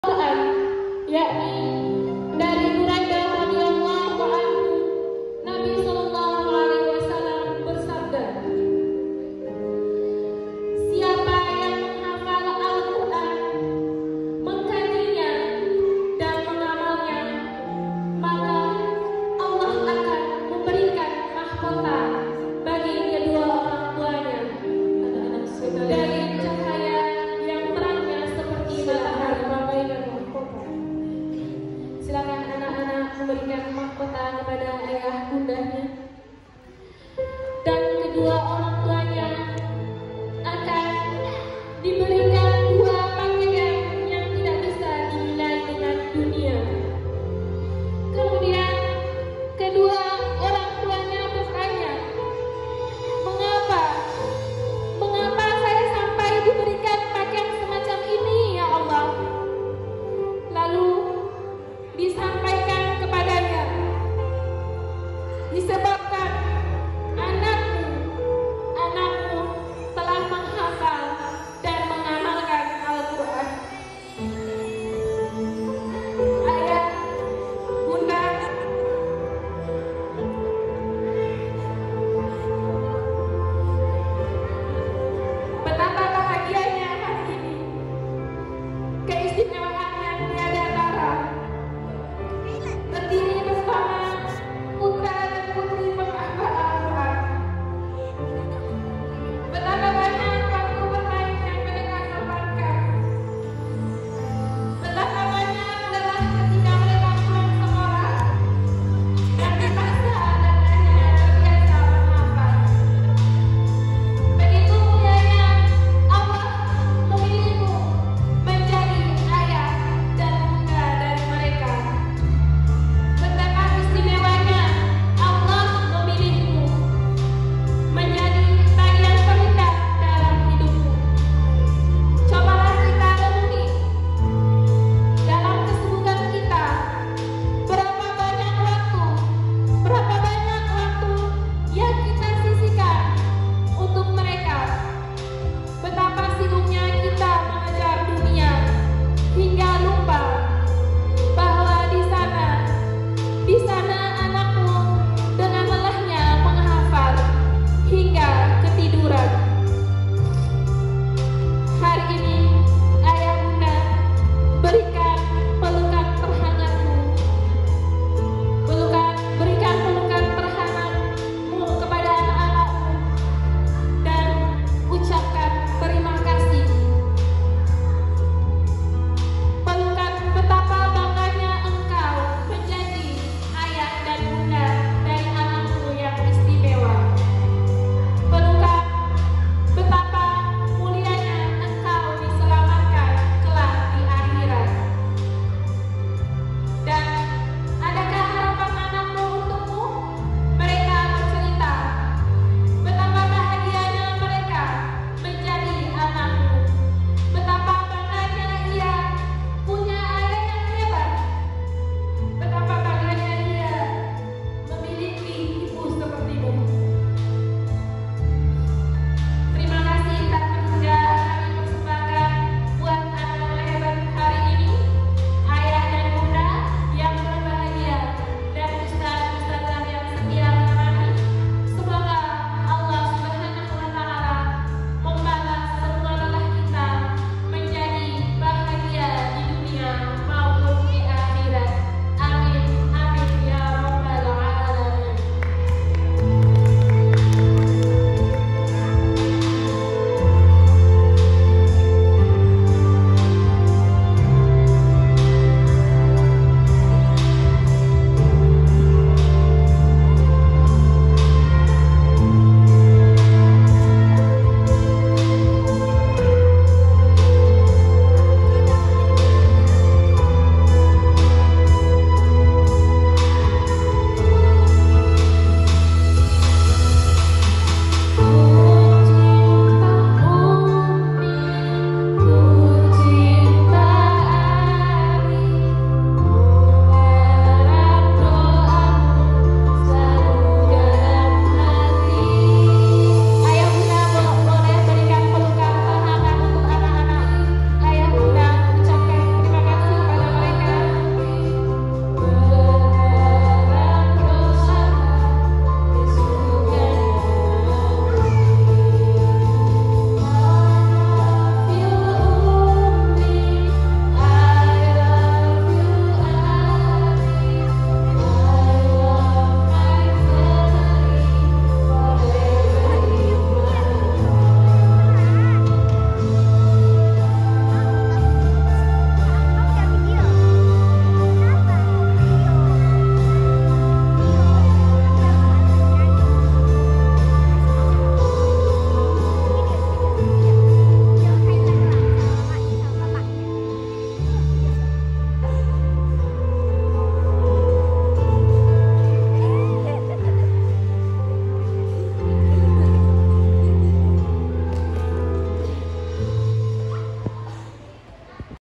MENGHARUKAN ! Prosesi penyematan mahkota kepada orang tua peserta wisuda tahfidz Qur’an ke-VII
Wisuda Tahfidzul Qur’an ke VII MI Darul Hijrah Madani